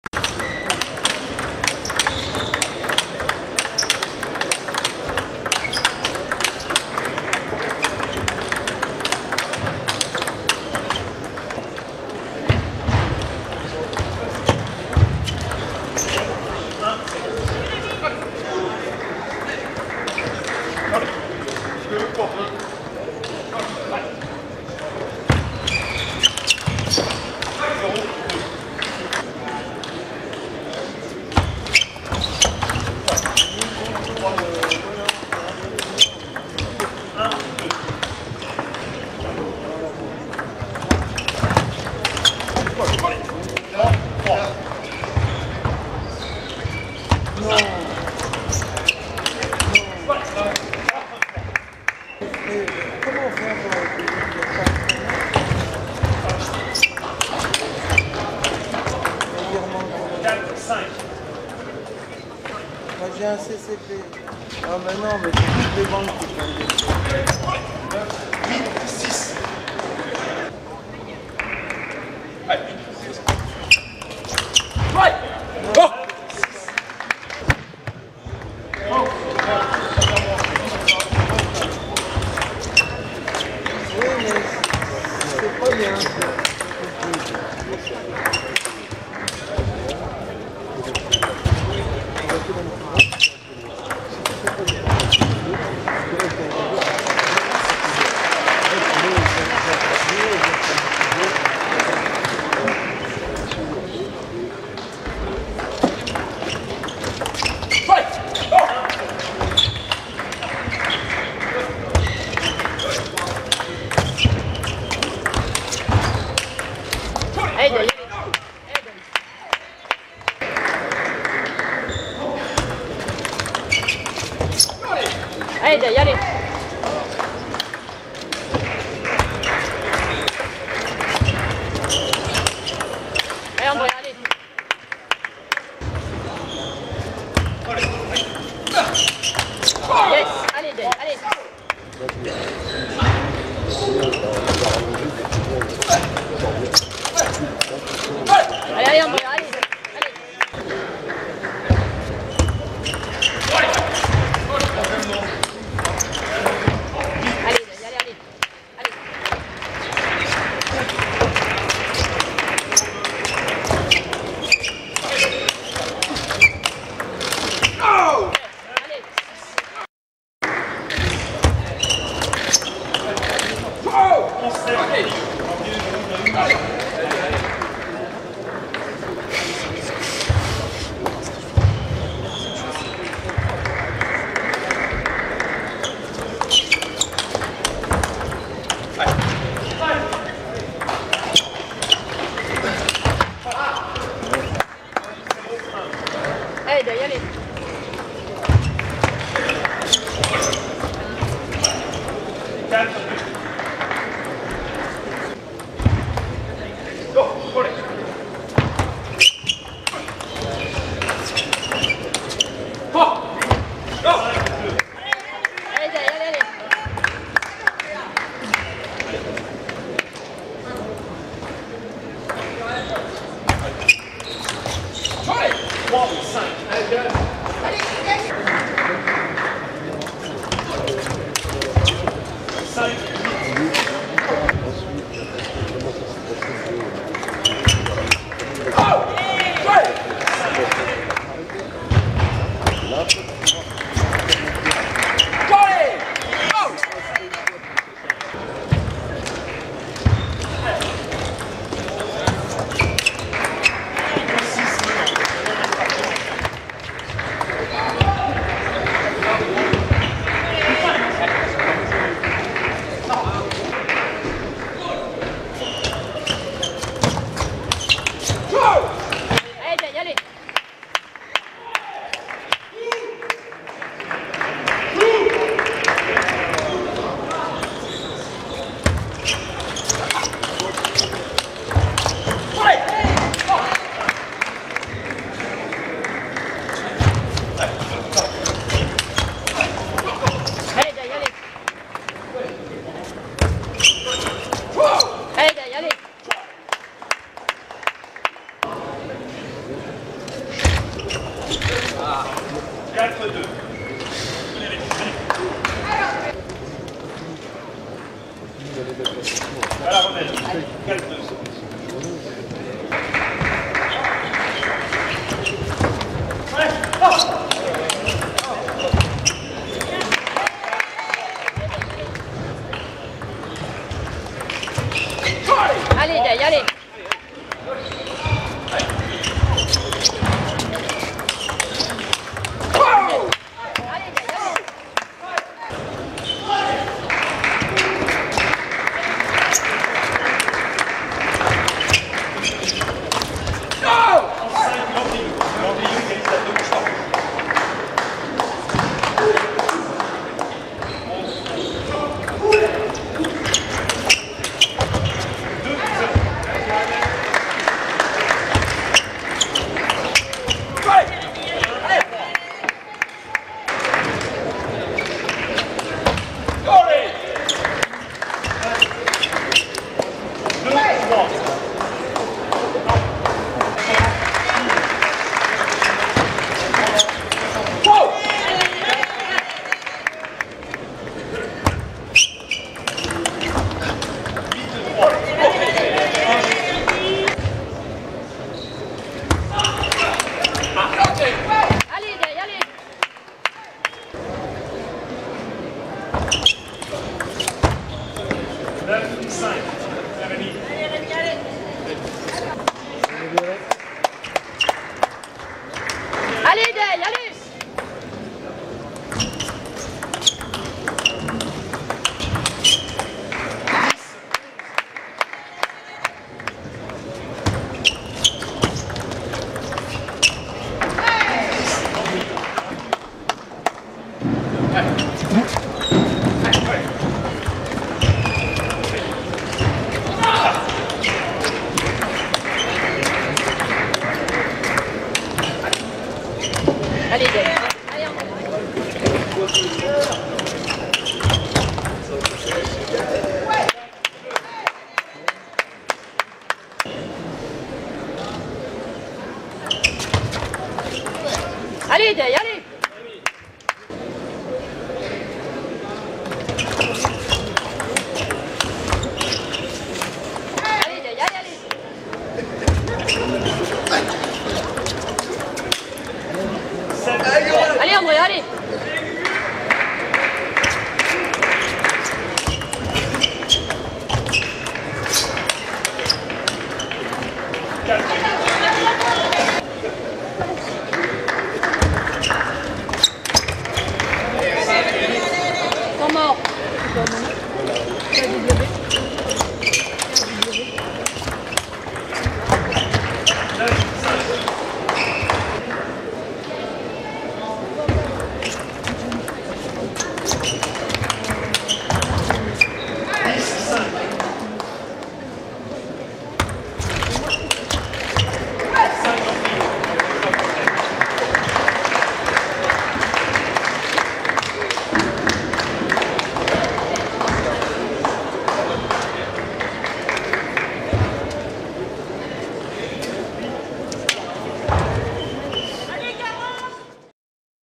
Andréa Rémi Menand France 2016 (momo rétro)
Match de qualification France 2016 Aréna de Brest le 15/04/2016